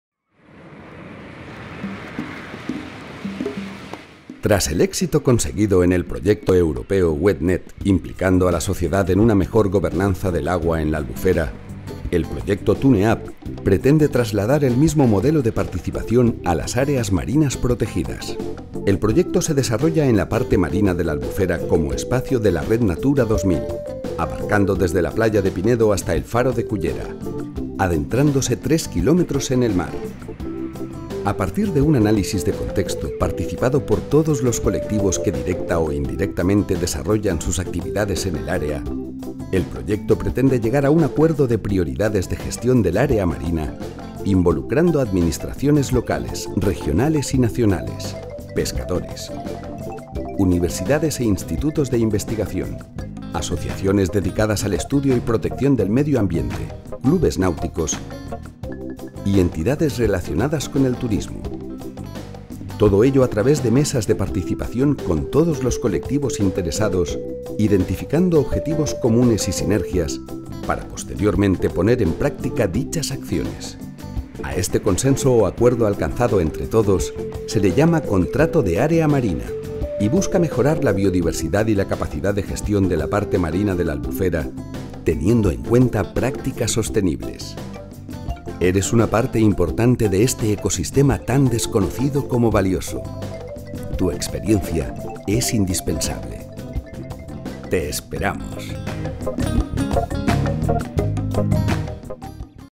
TUNE UP
El proyecto Interreg TUNE UP, cofinanciado por el Fondo Europeo de Desarrollo Regional, aborda la cuestión de la participación social en una mejor gestión de las áreas marinas y costeras del Mediterráneo, que son entornos interconectados vulnerables que gestionados adecuadamente contribuyen enormemente a la conservación de la biodiversidad, entrelazando aspectos científico-ambientales y las preocupaciones de gobernanza.